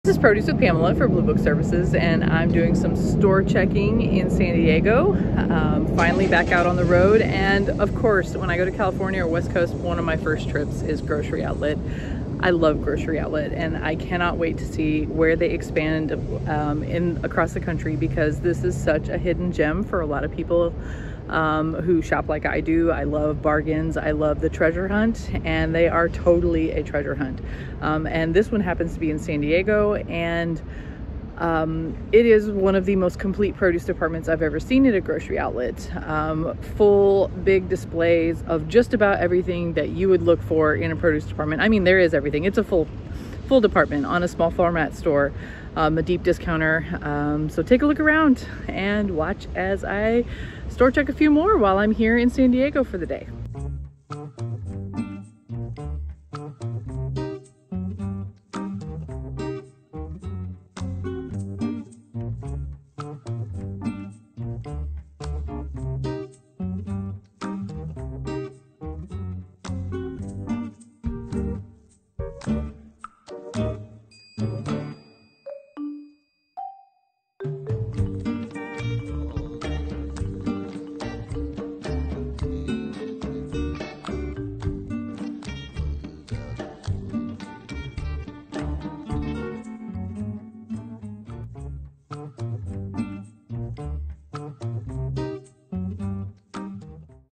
On the Road: Grocery Outlet, San Diego
I love a good Grocery Outlet store check. From the first time I saw the store in the Seattle area about eight years ago to today, Grocery Outlet has grown tremendously.

The "treasure hunt" style deep discounter operates 428 stores in the U.S., more than 50% of which are in California. The company announced plans to expand in new markets, and plans to open 10 new stores in the Mid Atlantic this year, and grow by about 10% each year.

The produce department is bigger than what you'd see at other discounters, with a lot more variety and flexibility in merchandising than I see at a store like Aldi or Save A Lot.

Grocery Outlet does a lot more opportunity buying than other stores, in my experience. Being in California, so close to where so much produce is grown, also helps foster these programs in stores.